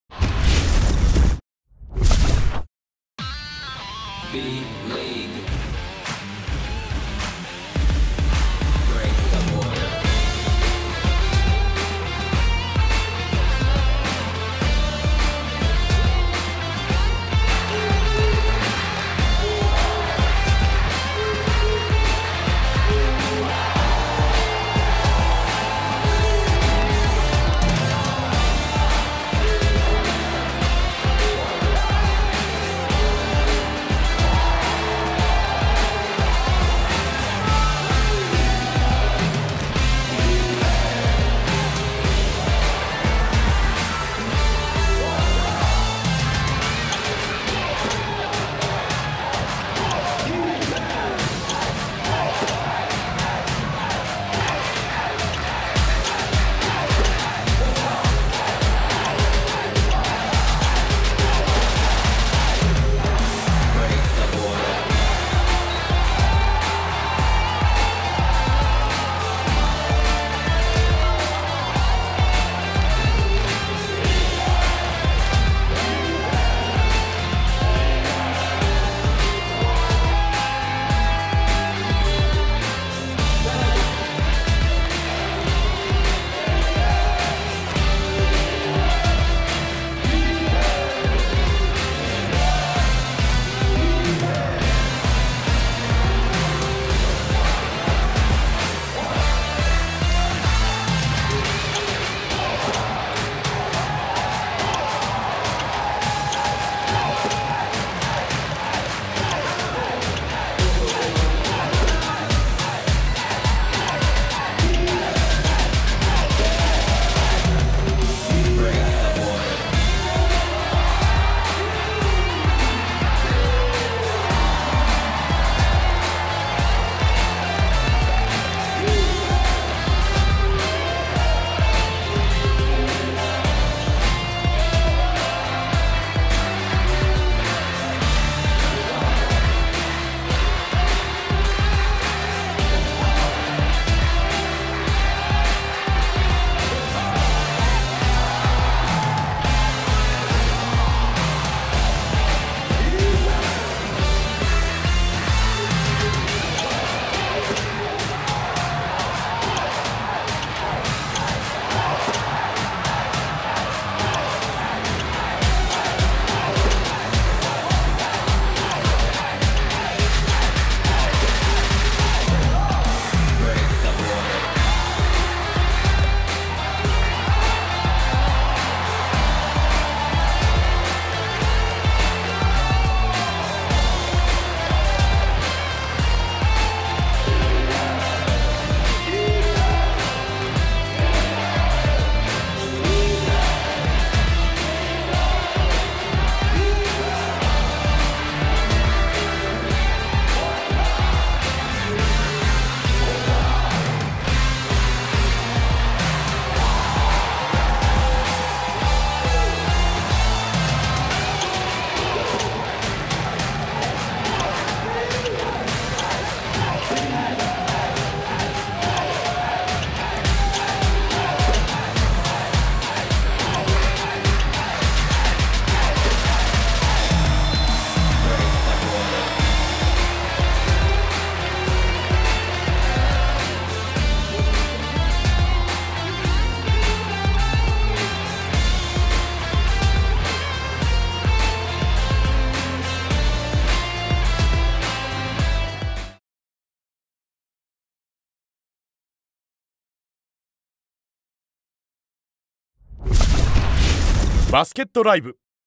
【B2ハイライト】03/10 信州 vs 東京Z（18-19 B2第25節）
B.LEAGUE B1 B2全試合配信

B.LEAGUE 03/10 第25節 信州ブレイブウォリアーズ(中地区) vs アースフレンズ東京Z(中地区) 試合会場：ことぶきアリーナ千曲